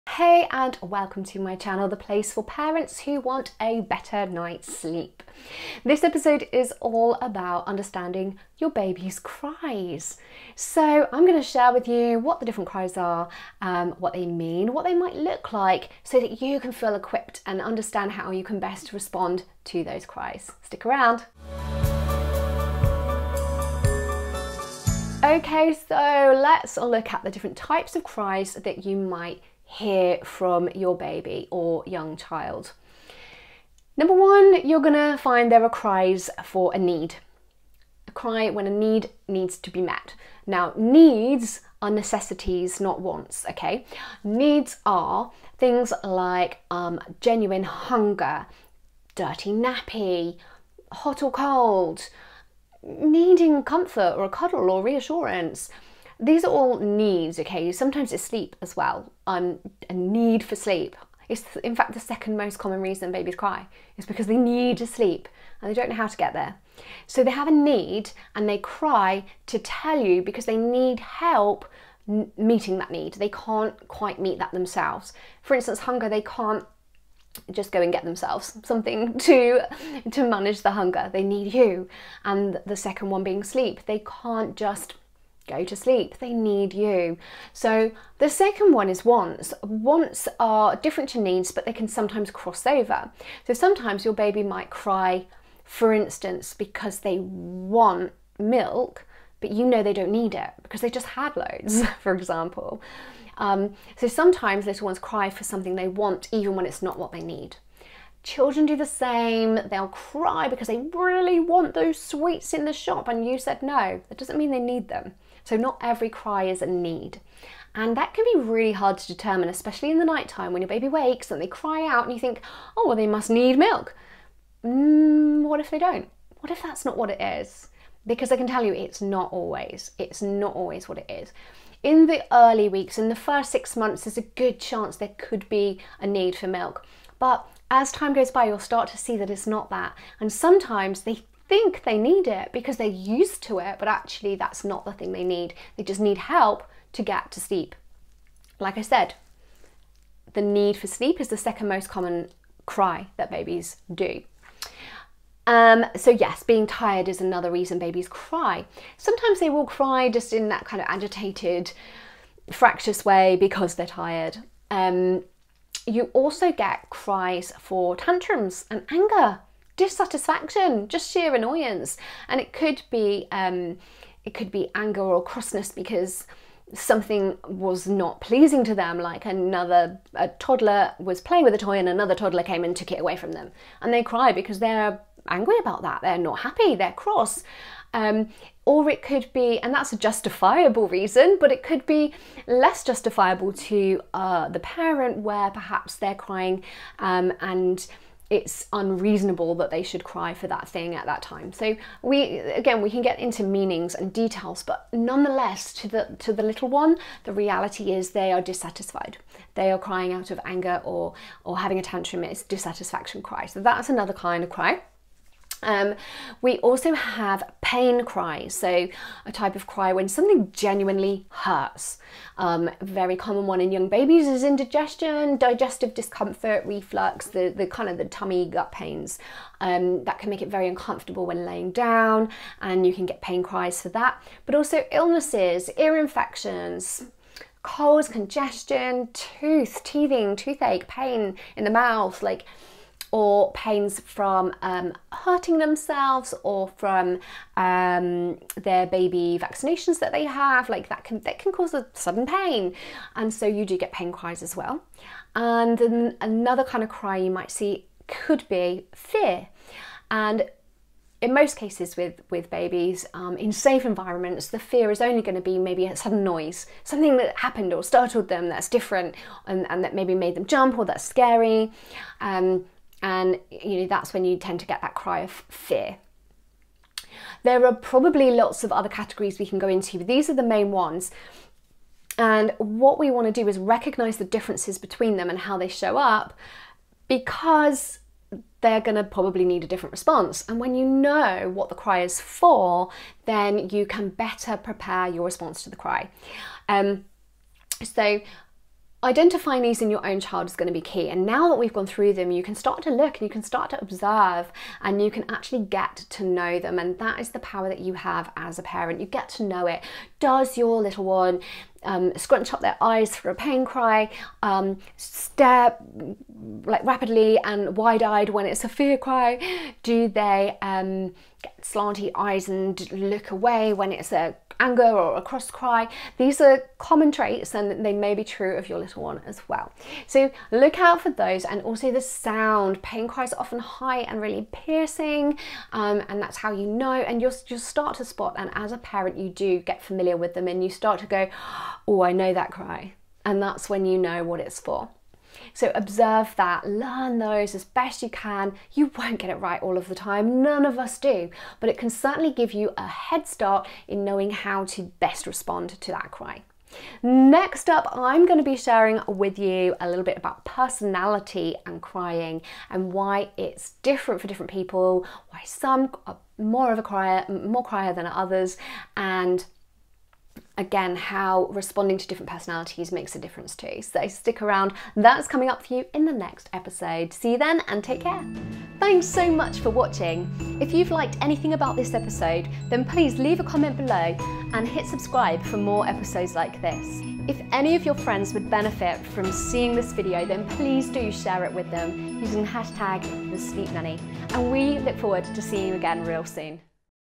154. Know Your Baby's Cries
In this video I'm going to share with you what the different cries are, what they mean, what they might look like, so that you can feel equipped and understand how you can best respond to those cries.